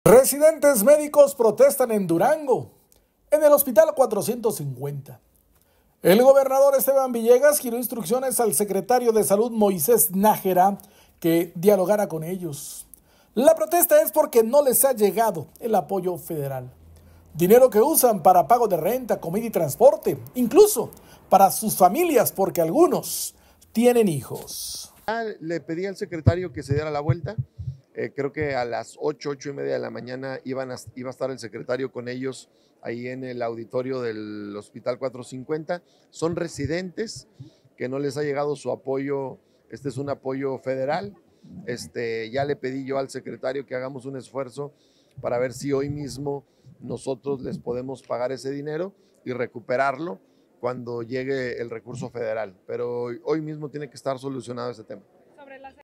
Residentes médicos protestan en Durango, no les llega el apoyo federal
-Residentes médicos protestan en Durango, en el Hospital 450; el gobernador Esteban Villegas giró instrucciones al secretario de Salud, Moisés Nájera, que dialogara con ellos.

La protesta es porque no les ha llegado el apoyo federal, dinero que usan para pago de renta, comida y transporte; incluso para sus familias porque algunos tienen hijos.